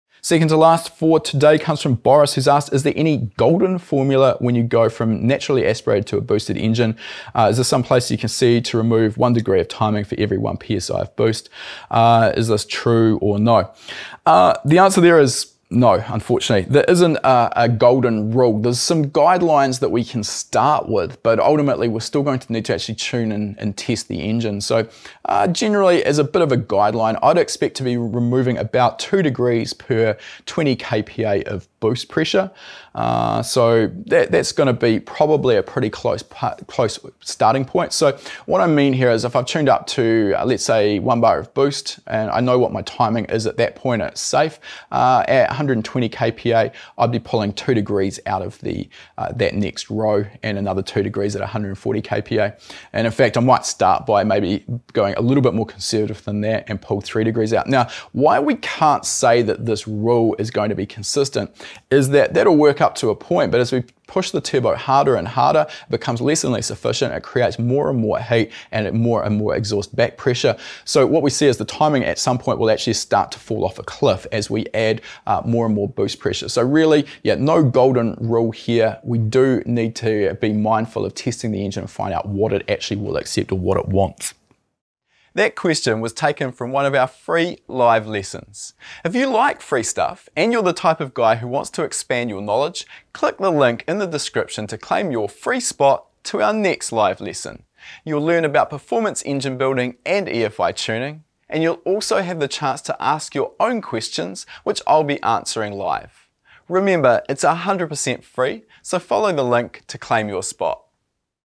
The Golden Formula? | NA To Boosted Application [HPA Q&A]
Is there a 'Golden Formula' that we can use to adjust our map when going from NA to forced induction/boosted application?

The quick answer is no, you need to retune the engine, but there are some guidelines you can follow to help you speed up that tuning process a little, however, unlike a 'Golden Formula' it still needs verification rather than being a set and forget method.

This was just one question taken from a free introduction to EFI tuning lesson.

🏎 Start learning how to EFI tune and watch the full lesson